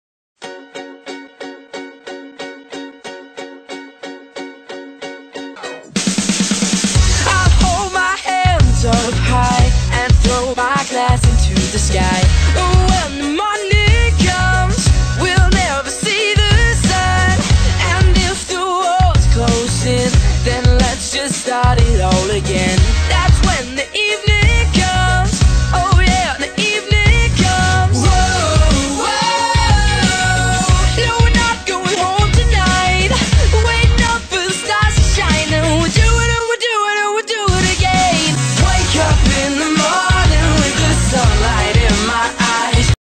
L A S T N I G H T
a quick vid of all my Recent meetups witth Holly (wildlikearider) Jade (TJEquestrian) and Megan (MCShowjumping) enjoyy:) xx